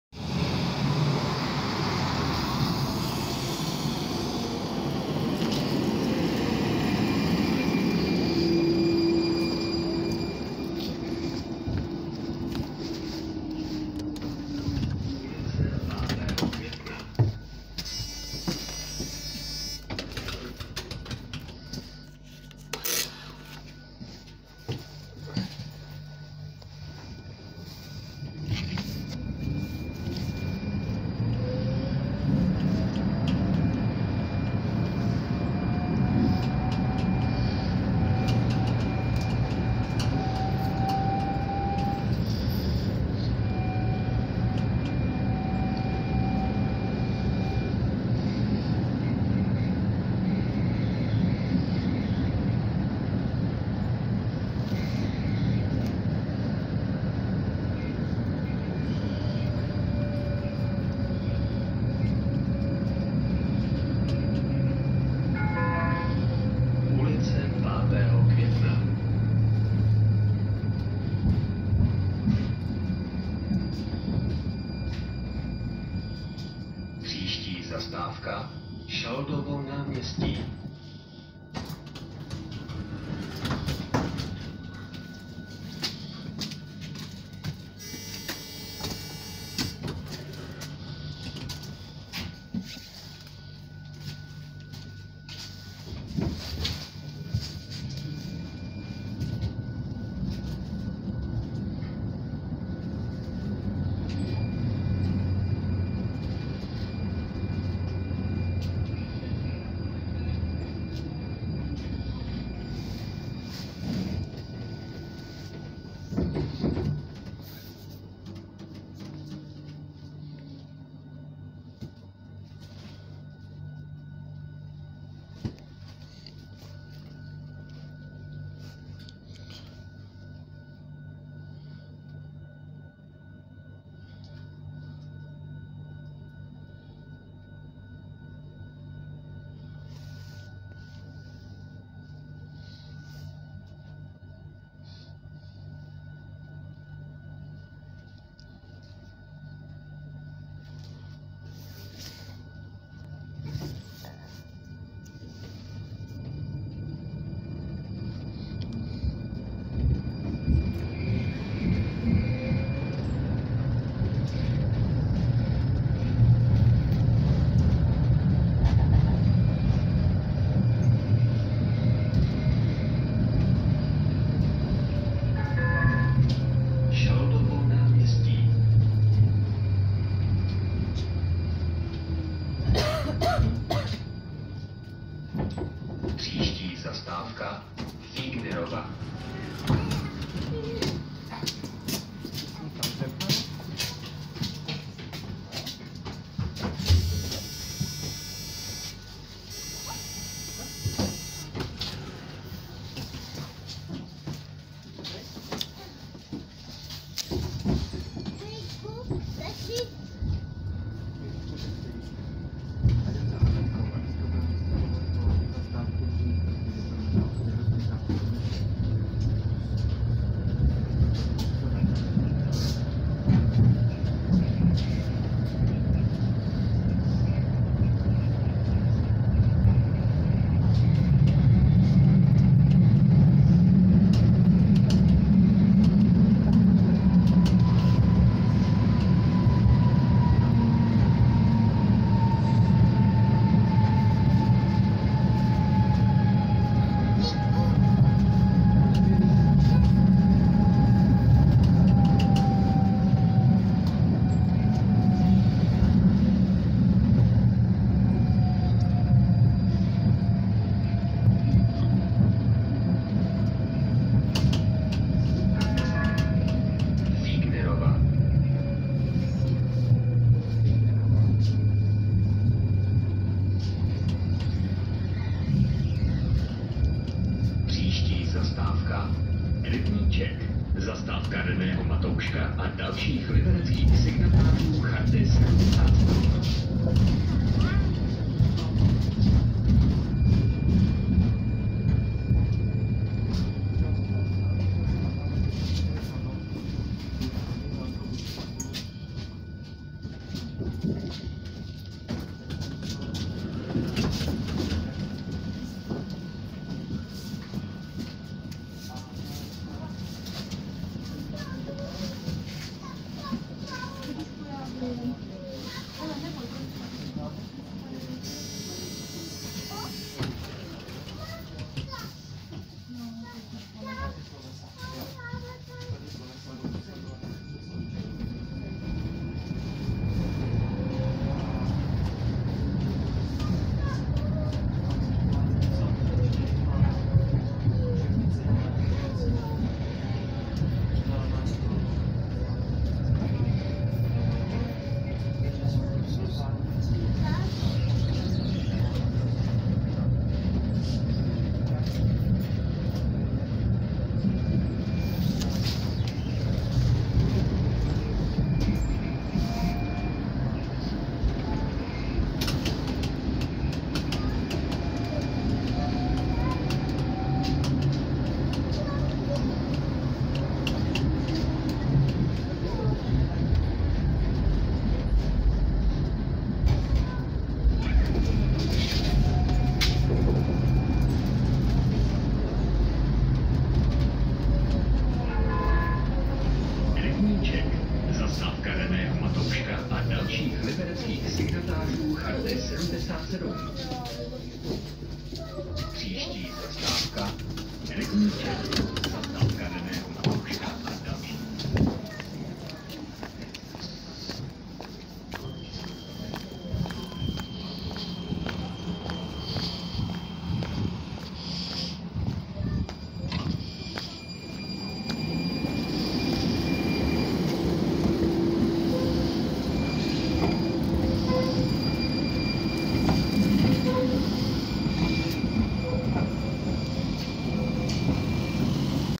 Jízda Libereckou tramvají T3M.04 u předních dveří
Jelo se z ulice Průmyslová škola do ulice Rybníček.

Linka tramvaje byla 2